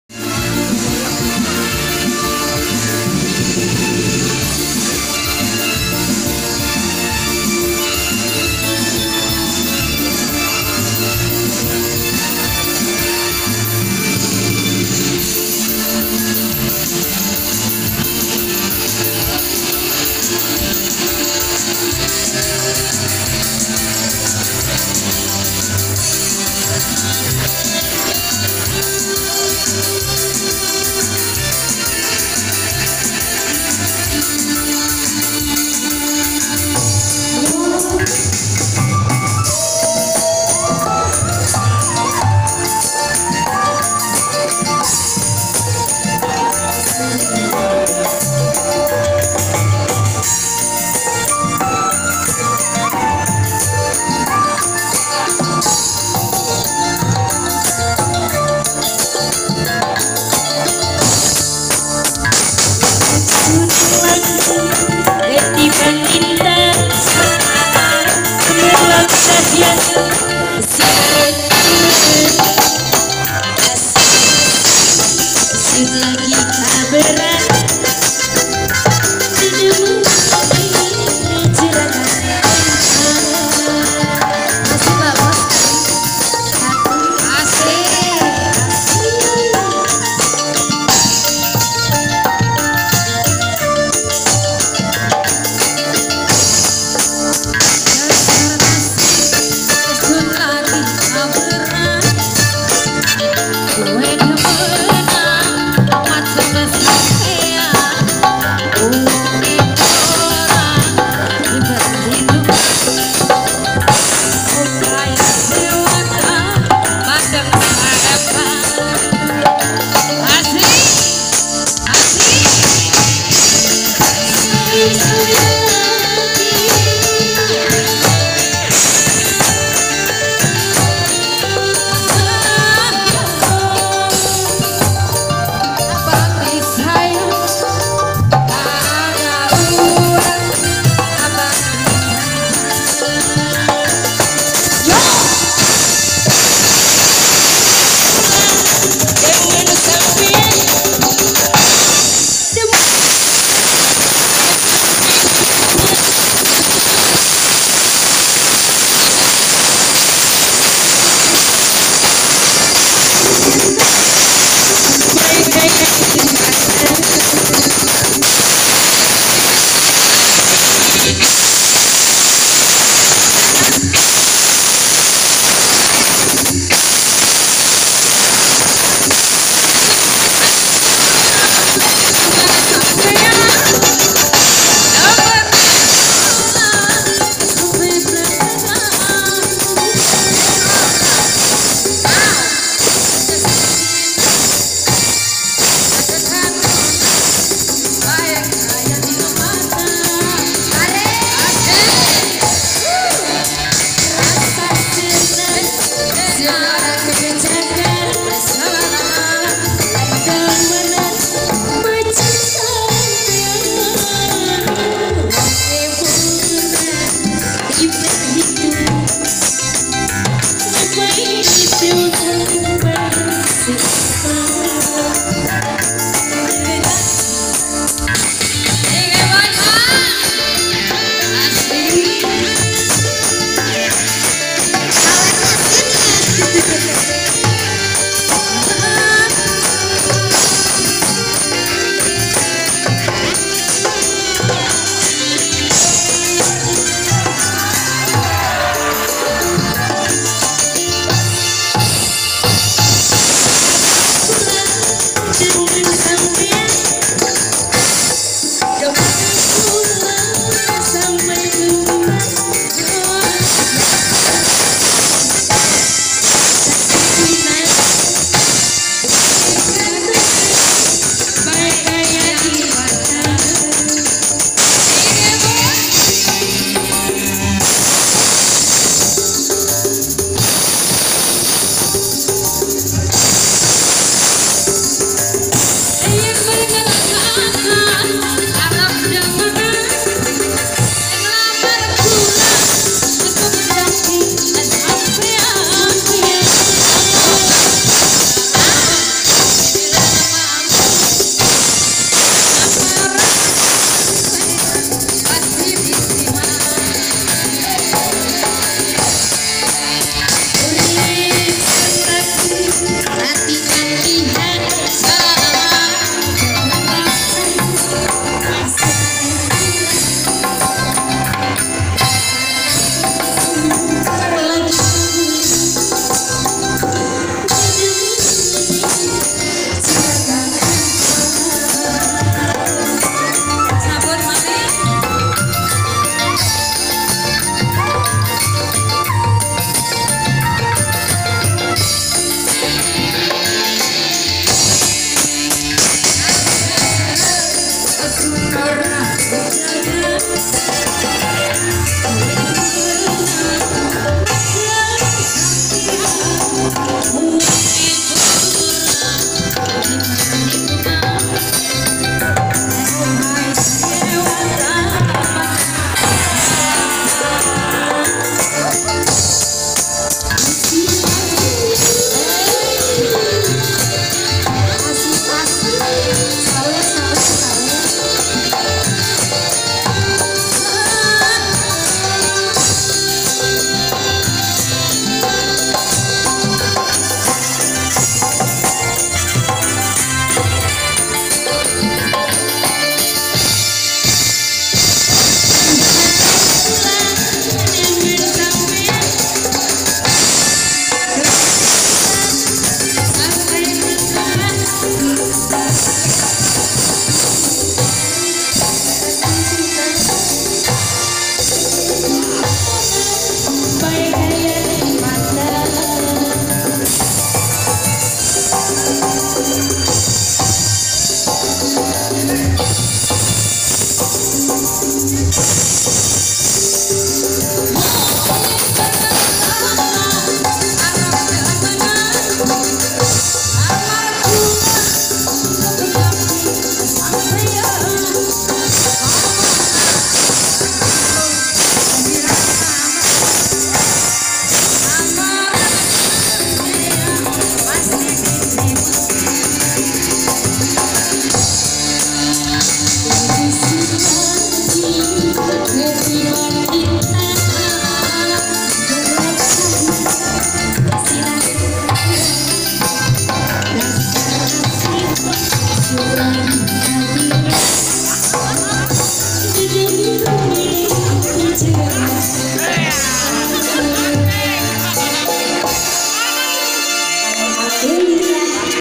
Kemeriahan acara HBH Jabar ngahiji di wisata kuda paku haji cimahi bandung
Hiburan setelah halal bi halal semua member komunitas charade G10 Indonesia dan dan komunitas2 lainya di wisata kuda paku haji cimahi bandung, joged2 biar tambah akrab lagi dngn sesama member lainya, semakin meriah ketika salah satu member dari G10 Indonesia chapter cirebon nyawer banyak banget,di panggung jadi hujan uang, mantaaaap...